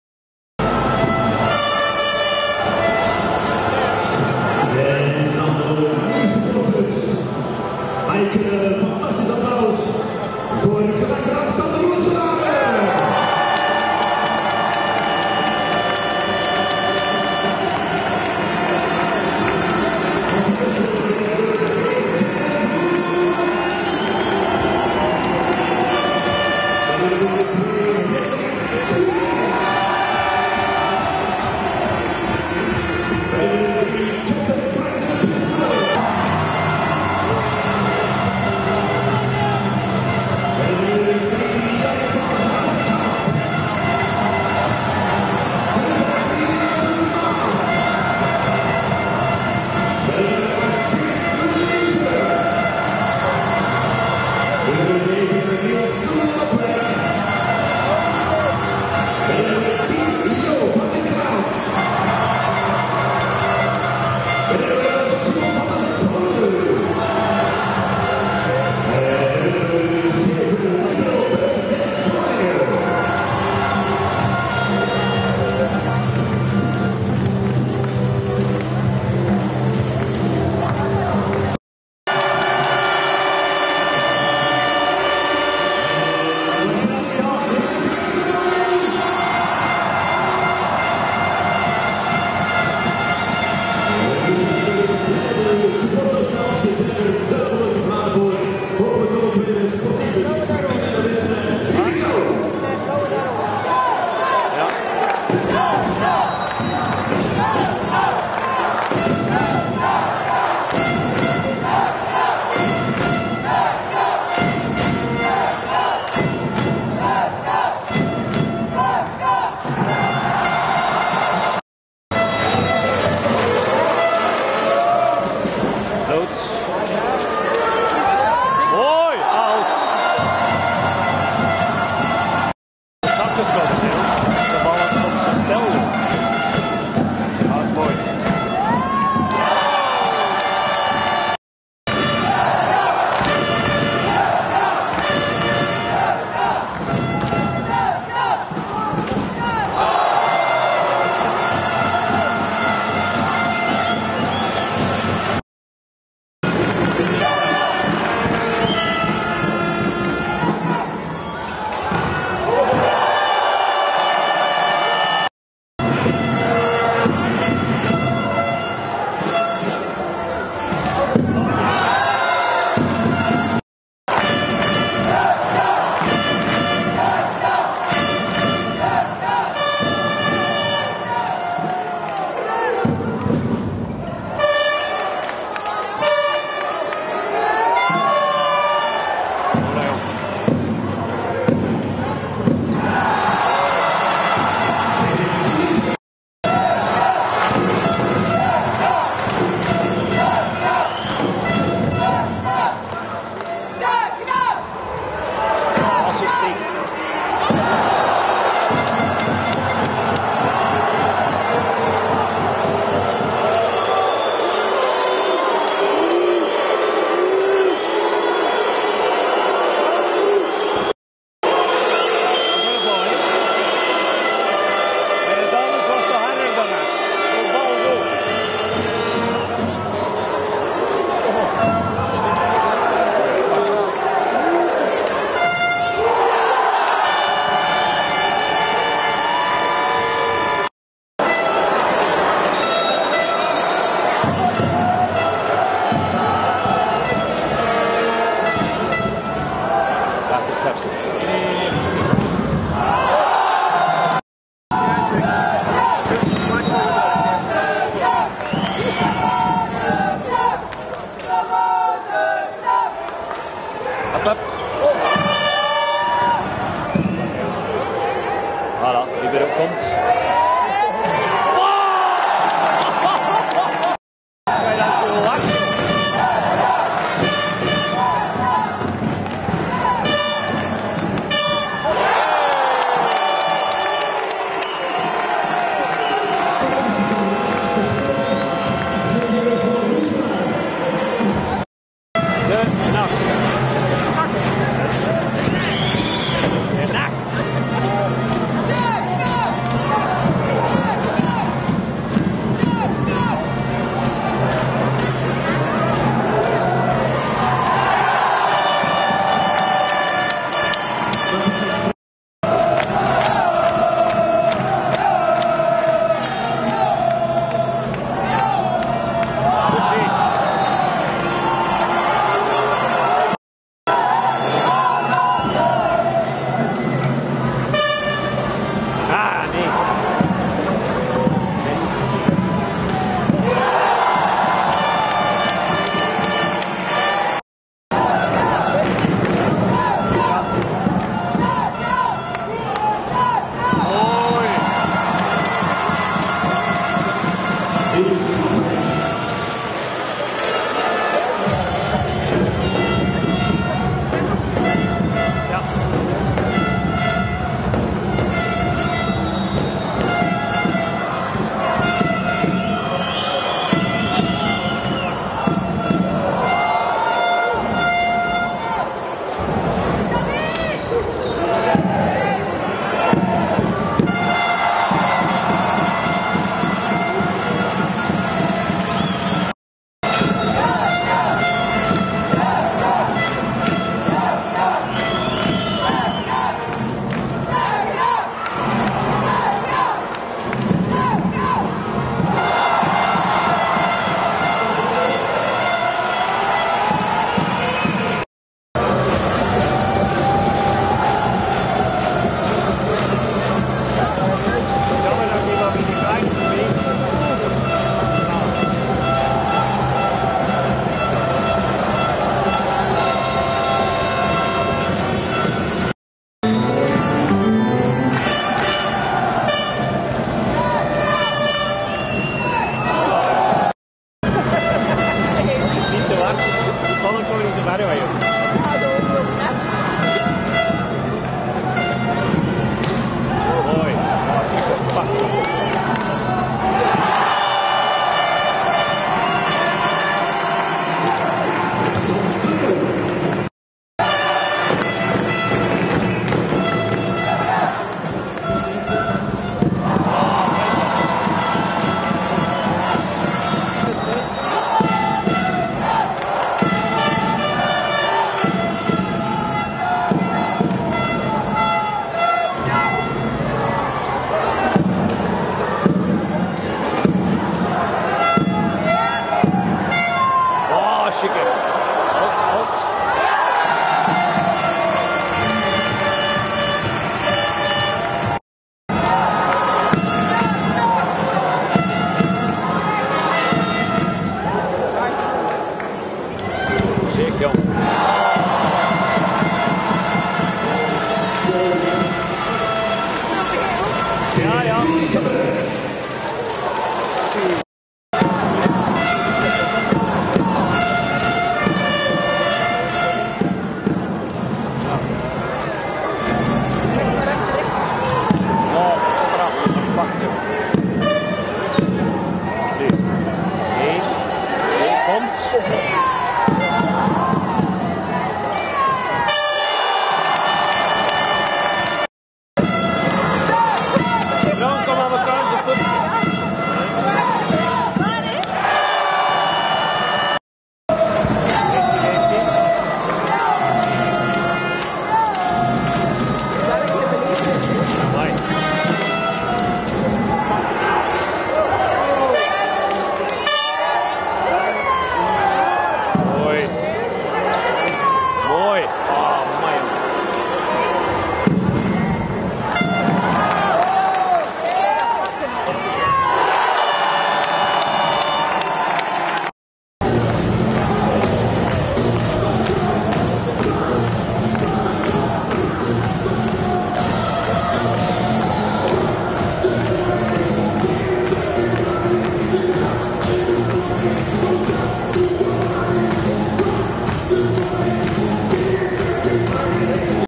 Finale 2011 Volleybal heren Noliko Maaseik - Knack Roeselare
Finale 2011 Volleybal heren Noliko Maaseik - Knack Roeselare. Eerste set